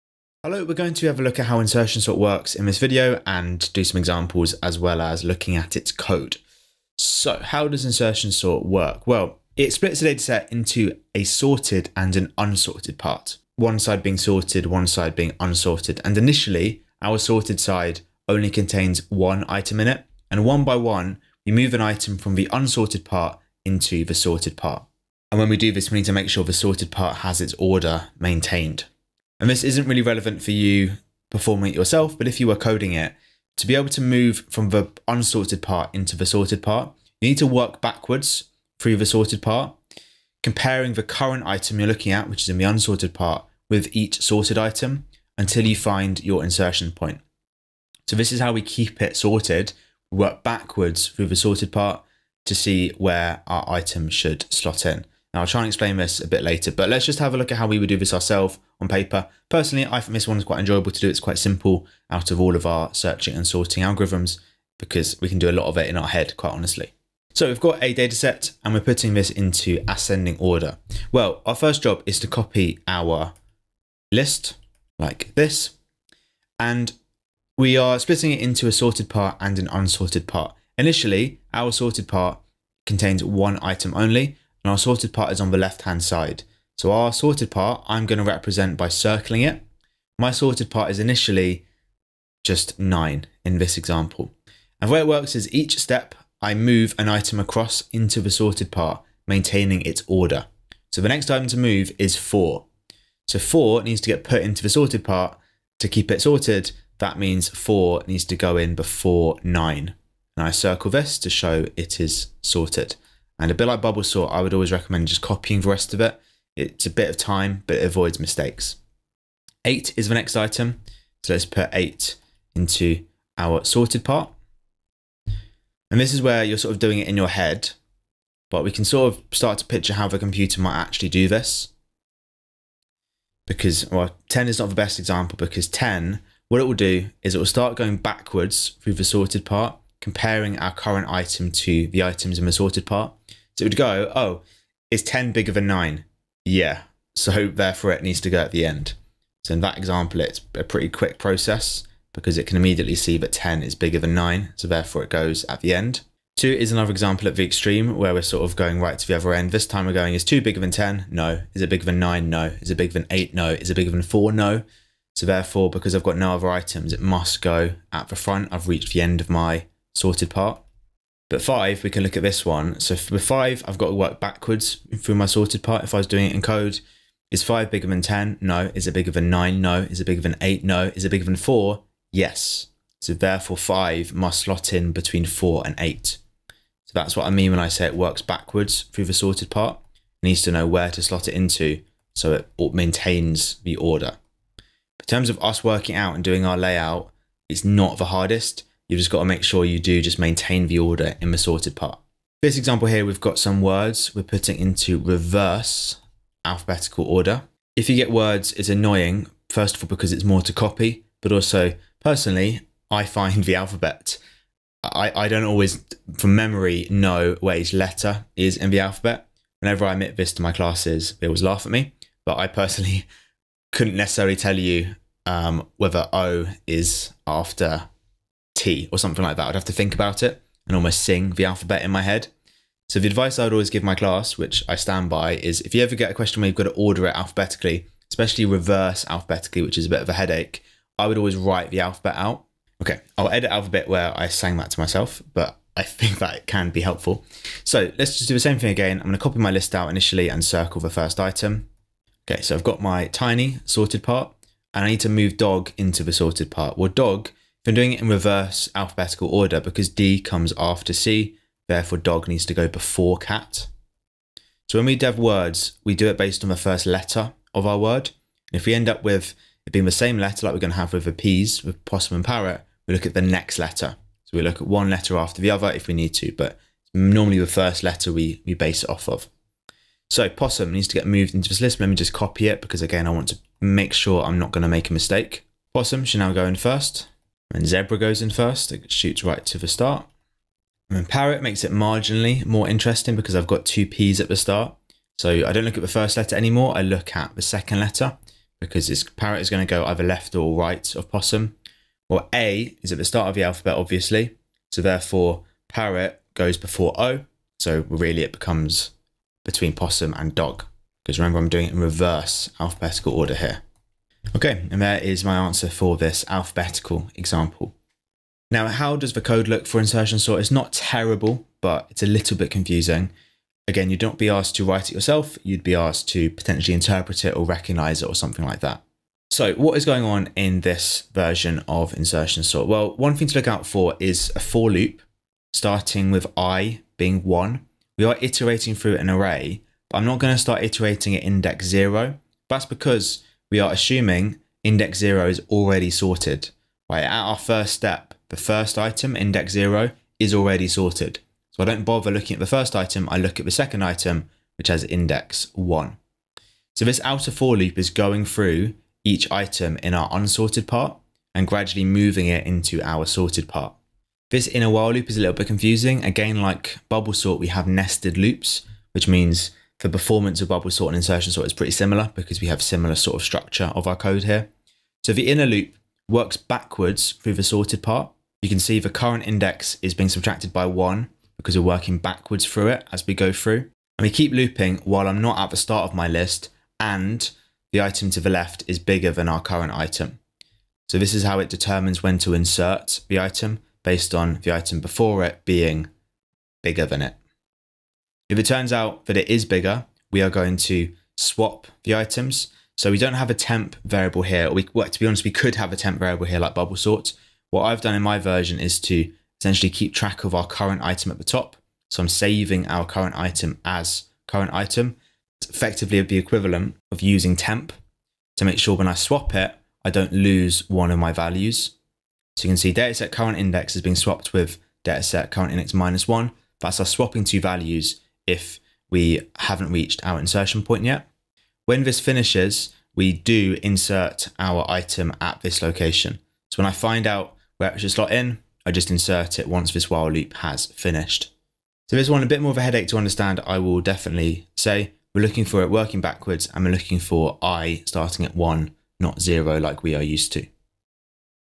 Insertion Sort
Looking at how the insertion sort algorithm works and how it can be applied (by hand) to a data set. The code is also shown and described.

Made for OCR GCSE Computer Science Paper 2 (J277).

0:00 How insertion sort works
0:58 Example #1
3:22 Example #2
5:19 Looking at the code